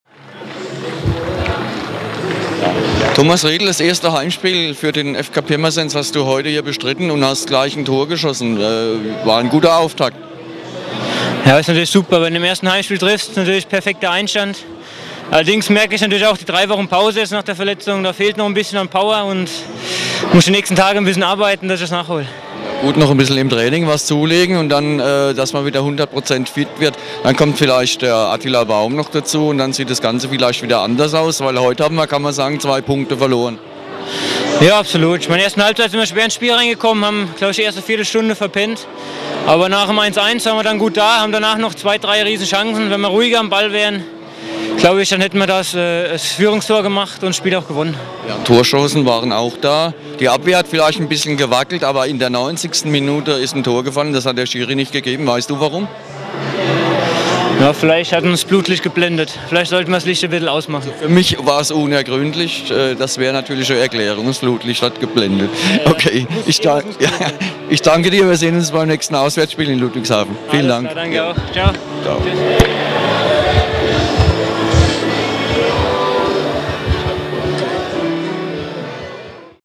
Interview mit Thomas Riedl
Interview mit FKP-Spieler Thomas Riedl nach dem Spiel der Oddset-Oberliga Südwest zwischen dem FK Pirmasens und dem SVN Zweibrücken am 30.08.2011